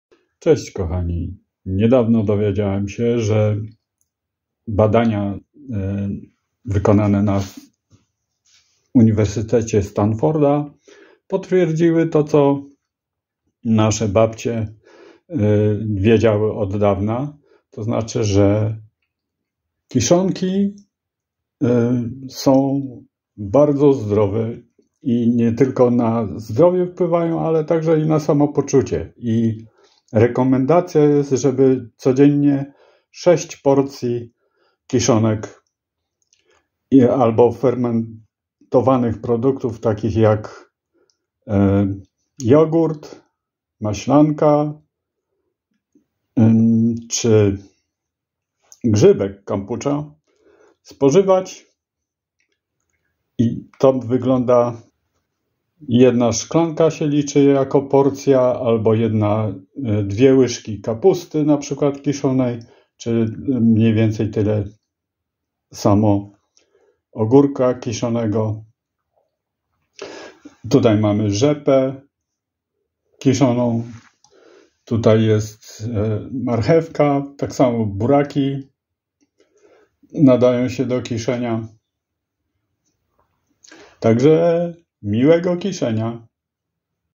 Sześć Porcji Dziennie! 😃
food kiszonka vlog Tao-Mola 20230712; Naturalne probiotyki: kiszona kapusta, kiszone ogórki, kefir, jogurt, maślanka, kimchi itp. 😉